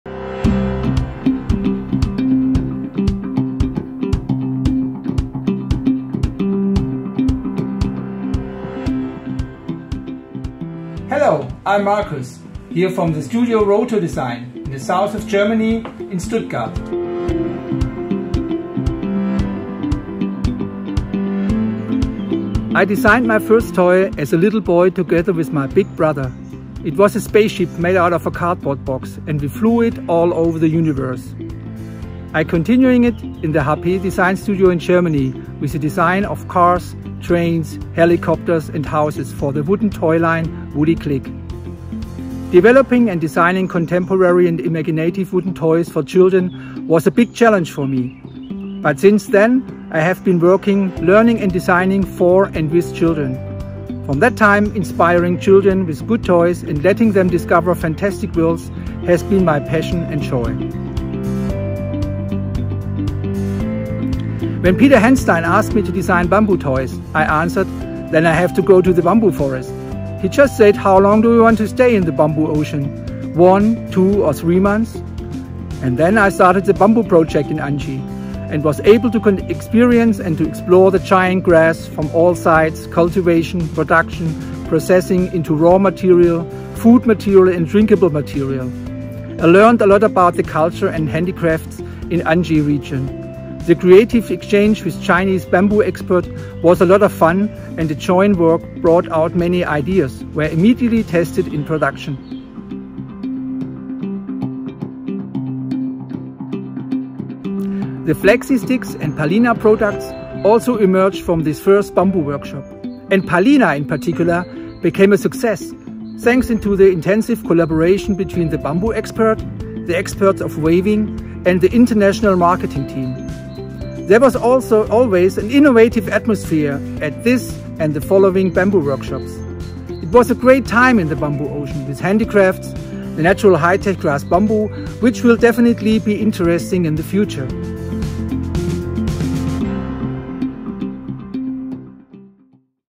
Designer's talk | Markus Hirche | Hapetoys
Hape has so many excellent products, which are inseparable from professional toy designers. Let’s learn about their behind-the-scenes stories!